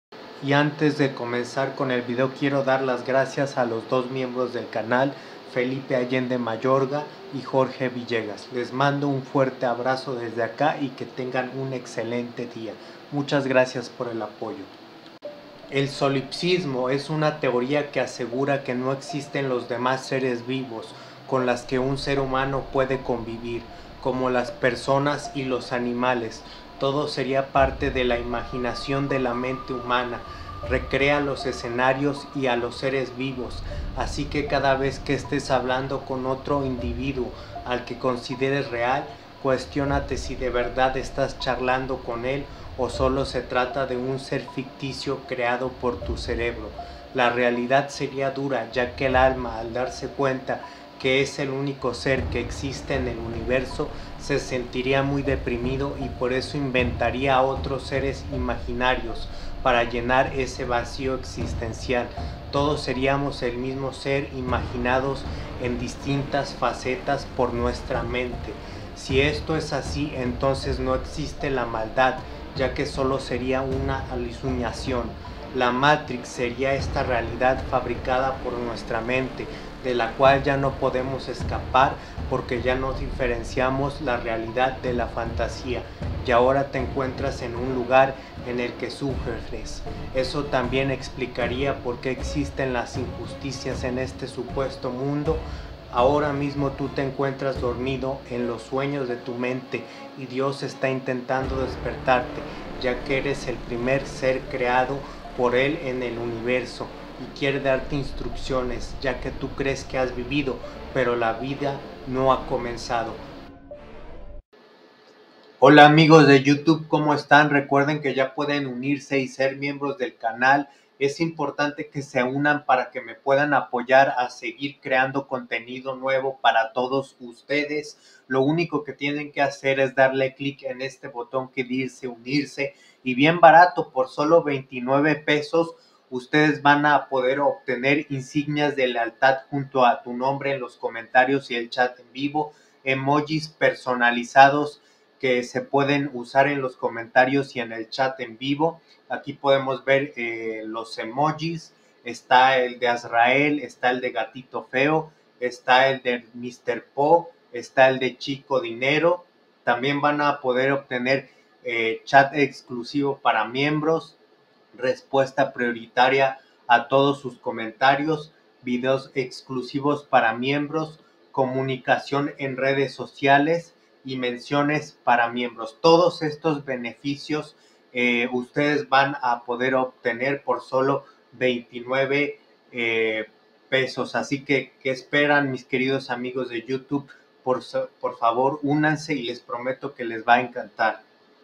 VIVIMOS EN UN SUEÑO LA TEORIA DEL SOLIPSISMO
VIVIMOS EN UN SUEÑO EXISTE ADEMAS DE LAS TEORIAS DE LA MATRIX EXISTE UNA FILOSOFIA LLAMADA SOLIPSISMO LA CUAL ASEGURA QUE LOS DEMAS SERES HUMANOS Y ANIMALES NO EXISTEN SINO QUE SERIAN IMAGINADOS POR NUESTRAMENTE Y QUE ASU VEZ ESTOS SERIAN TU IMAGINANDOTE EN DISTINTAS FASETAS ESO TAMBIEN EXPLICA PORQUE VIVIMOS EN UN MUNDO DE INJUSTICIA EL ALMA AL DARSE CUENTA QUE ES EL UNICO SER EXISTENTE EN EL COSMOS HABRIA DECIDIDO CREAR OTROS AVATARES QUE PERTENESEN ASU PROPIA ALMA ESTAMOS EN LA MATRIX UNA REALIDAD QUE NO ES LA VERIDICA Y QUE YA NO PODEMOS DIFERENCIAR LA FANTACIA DE LA VERDAD Y NO SOTROS JAMAS HABRIAMOS VIVIDO UNA VIDA FISICA ENREALIDAD PERO ABRIA UN SER PREOCUPADO POR DESPERTARNOS UNA CONCIENCIA SUPERIOR DIOS EL CUAL ESTA INTENTANDO DESPERTARNOS DE NUESTRO SUEÑO PARA EMPEZAR A VIVIR UNA VIDA PLENA Y VERDADERA EL PROBLEMA ESQUE SOMOS ENGAÑADOS POR NUESTRA PROPIA MENTE Y NOS ES CASI IMPOSIBLE SALIR DE AQUI HAHORA DEBES CUESTIONARTE CADAVES QUE HABLES CON UNA PERSONA SI ES REAL O NO
Únete a este canal para acceder a sus beneficios: